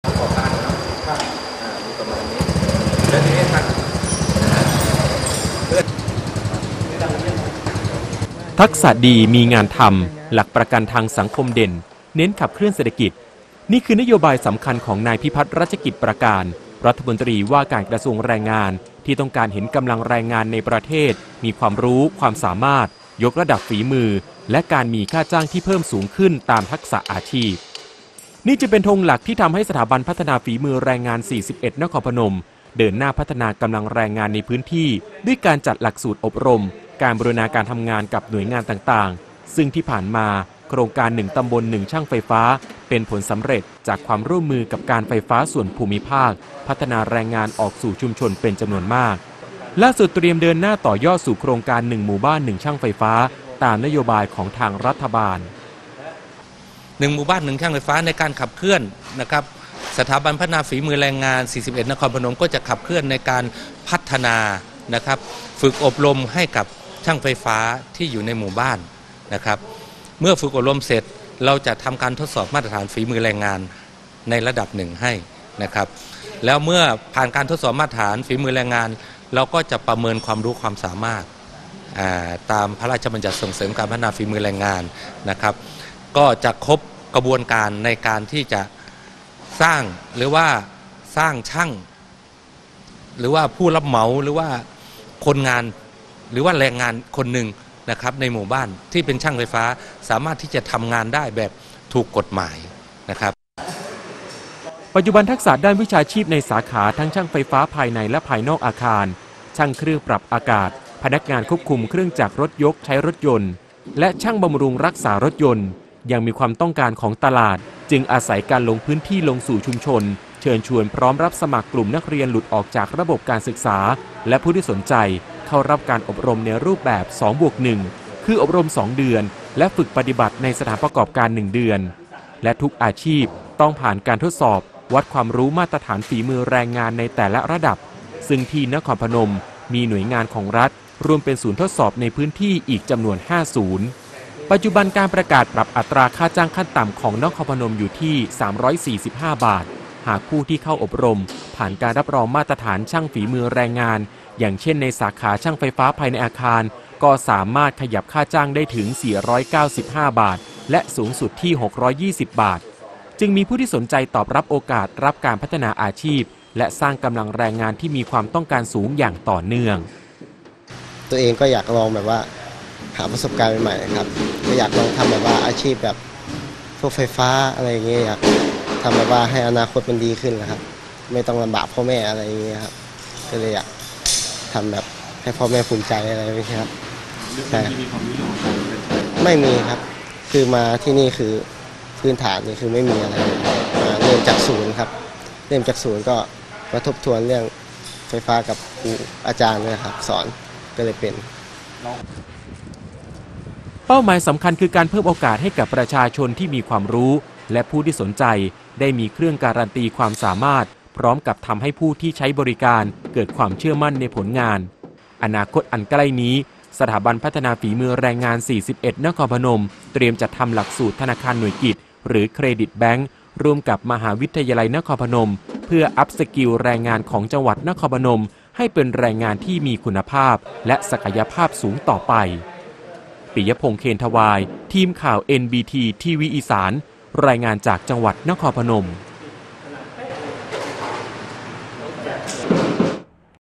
รายงาน "นครพนม เพิ่มทักษะมาตรฐานสร้างฝีมือแรงงานอย่างอาชีพ" ข่าวเช้า วันที่ 30 กรกฎาคม 2567
ความต้องการของอาชีพที่ต้องใช้แรงงาน ปัจจุบันมีความต้องการที่เพิ่มสูงขึ้น ที่สำคัญการจะมีแรงงานที่มีทักษะประสบการณ์จำเป็นต้องได้รับการทดสอบวัดมาตรฐานฝีมือแรงงานแต่ละกลุ่มอาชีพ เช่นเดียวกับที่จังหวัดนครพนม ได้ให้ความสำคัญในเรื่องนี้ ติดตามรายละเอียดได้จากรายงาน